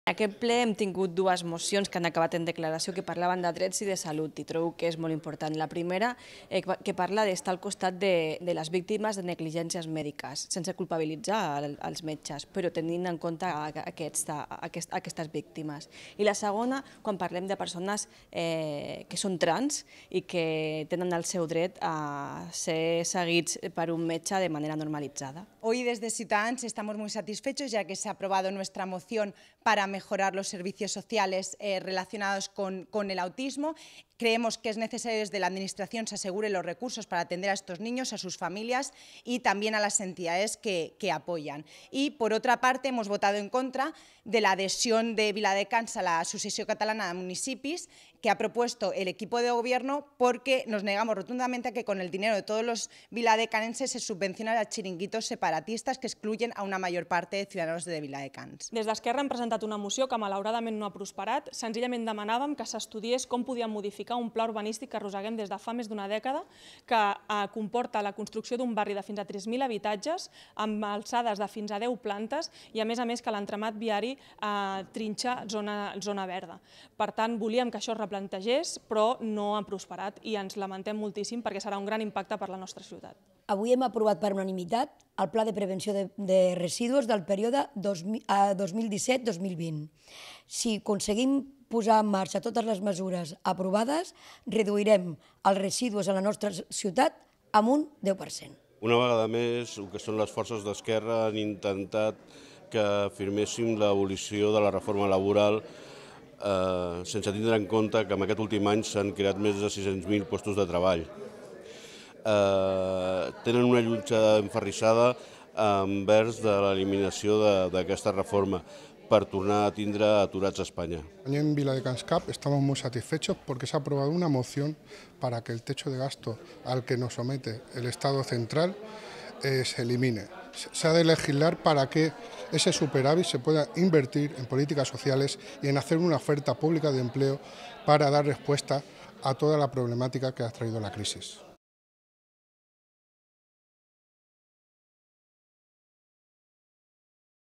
Opinions Polítiques del Ple Municipal de maig de 2017
Els i les portaveus dels grups municipals del Ple destaquen diferents punts de la sessió plenària.